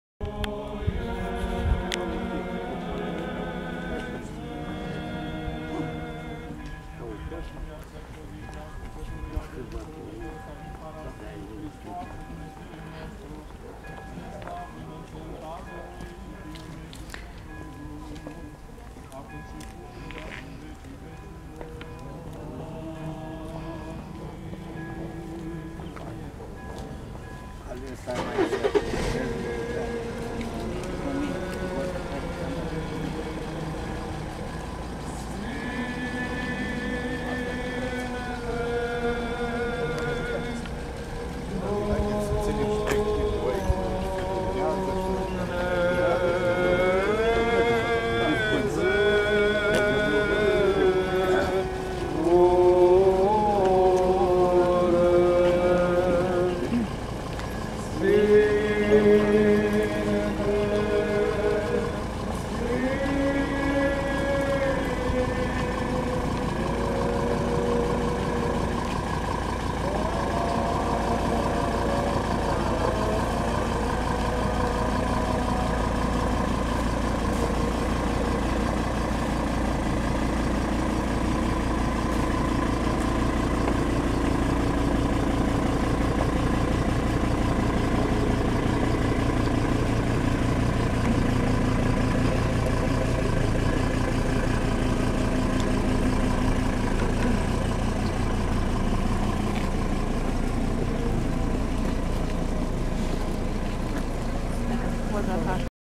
Înmormântarea liceanului Sabin Șerban, fiu de colonel SRI, ucis în propriul apartament
Sabin Paraschiv Şerban, elevul de la Liceul de Informatică găsit de tatăl său înjunghiat în casă, a fost condus pe ultimul drum de către familie şi colegi. Zeci de coroane funerare şi aproximativ 400 de persoane au participat începând cu ora 11 la slujba de înmormântare de la Cimitirul „Buna Vestire din CUG”. Slujba a avut loc în linişte, discret.